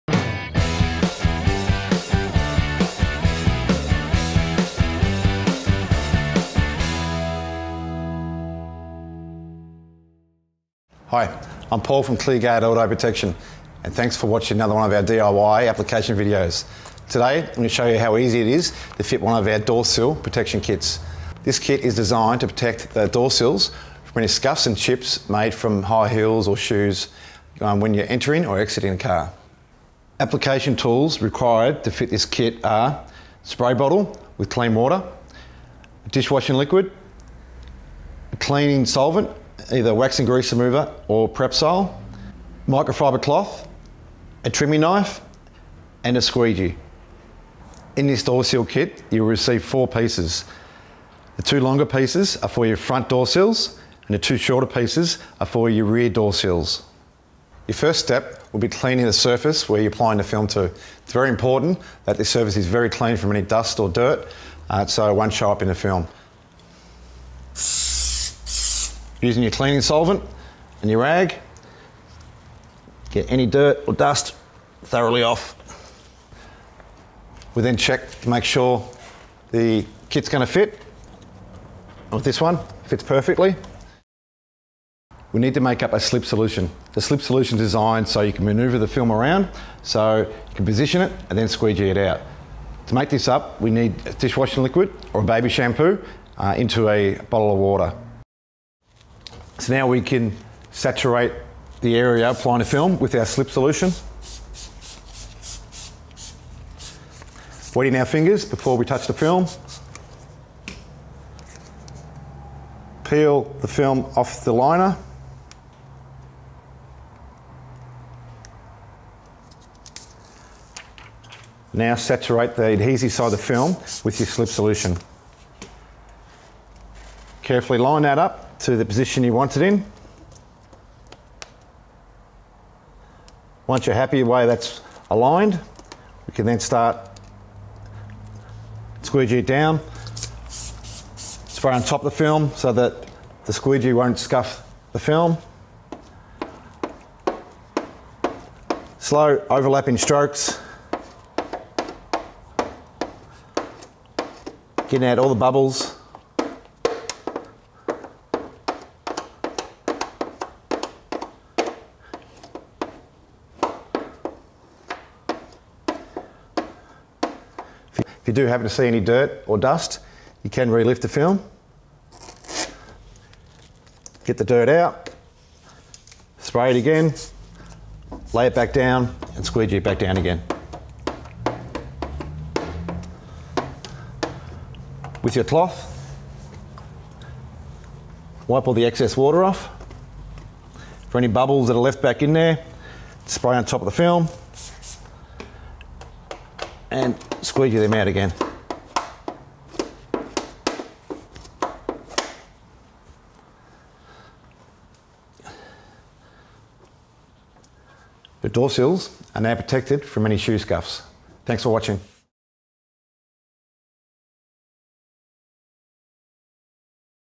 Cleargard 3M Door Sill Protection Film Installation
Protect your Door sills from scuffs & damaged caused while getting in & out of the vehicle. Made from 3M's  tough clear polyurethane Paint Protection Film. Once applied this material is virtually invisible.